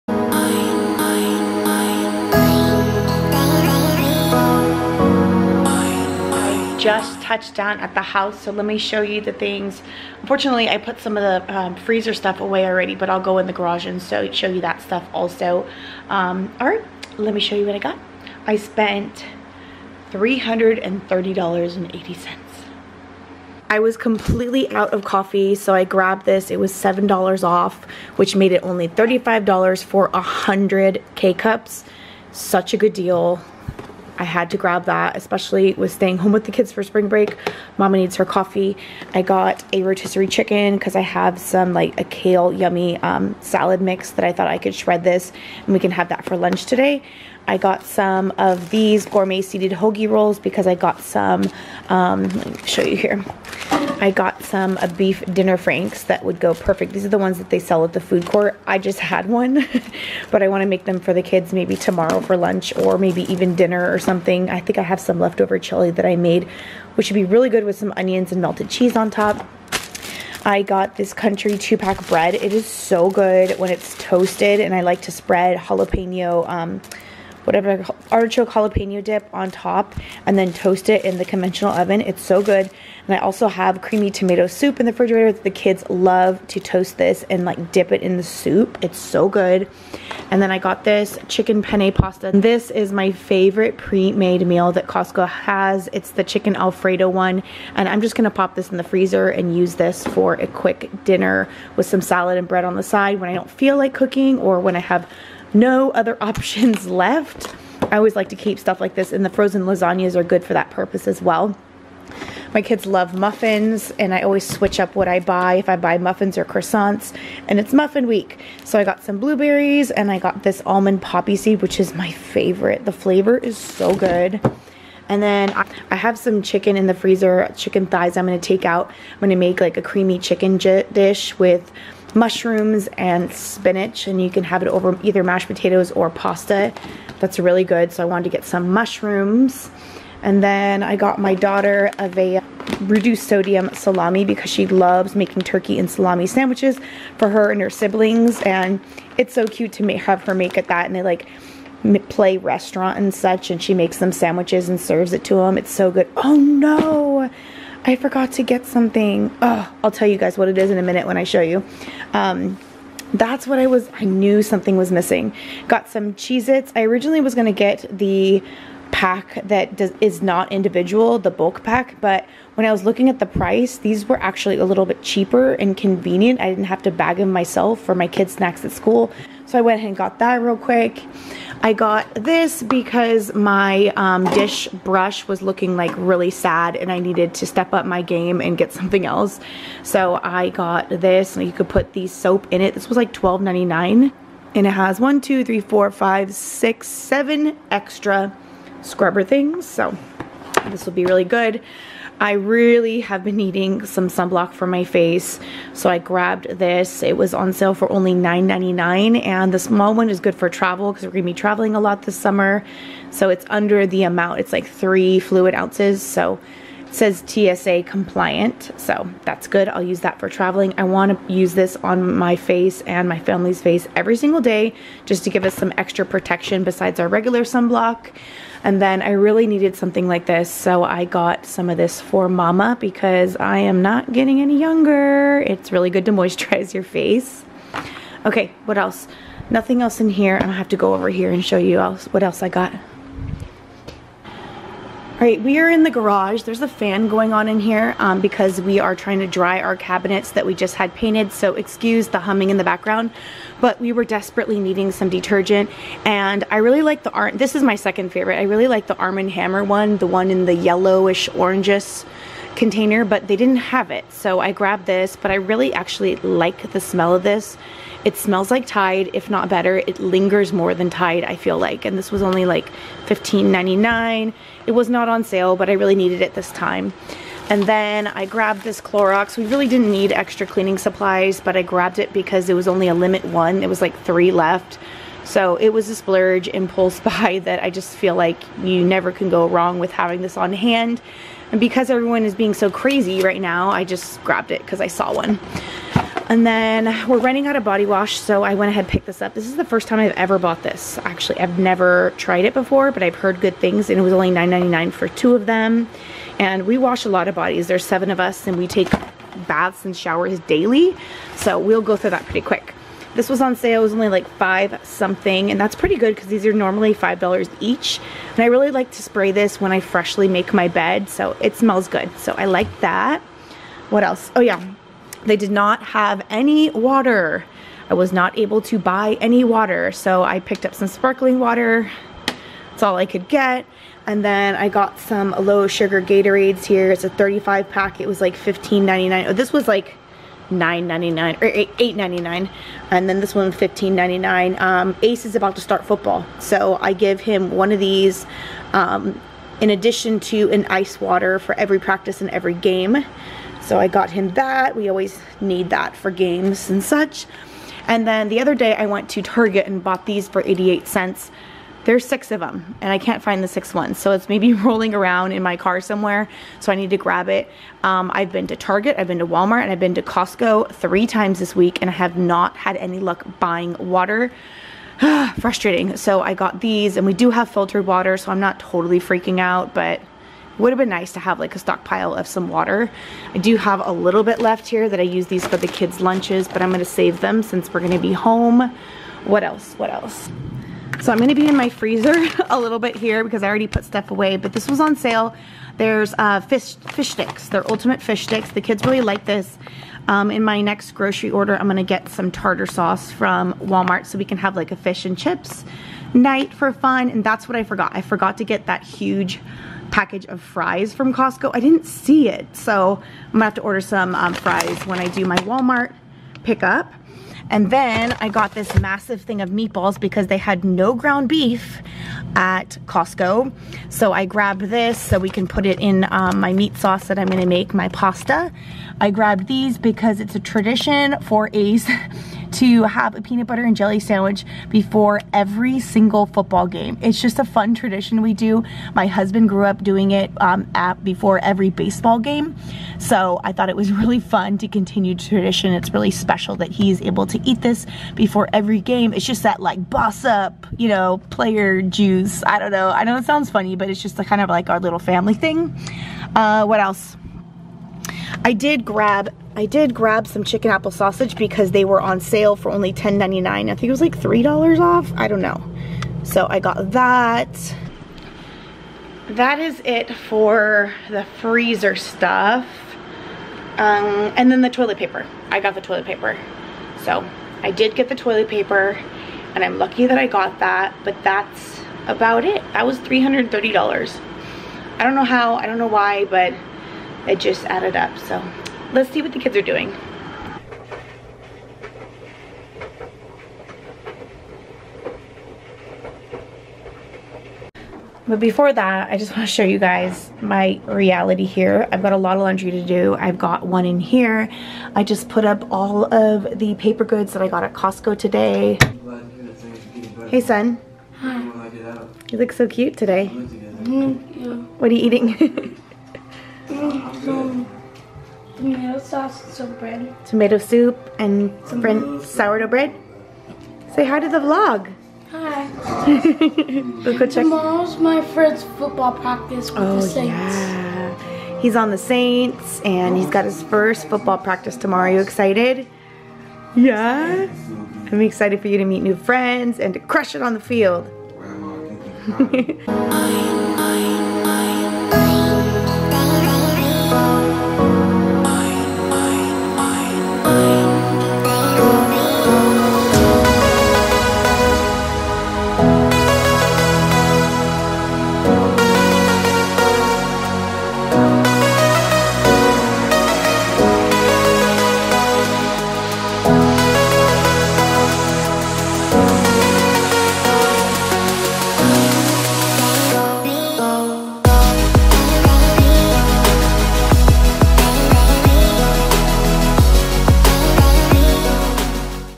What I Got At Costco For $330!! Costco Haul Prep for Quarantine //Large Family Costco Haul 2020
Check out what I got at Costco to prepare for this virus quarantine. Thank you all for being so supportive to my family and I. We appreciate each and every one of you.

Costco Haul Prep for Quarantine //Large Family Costco Haul 2020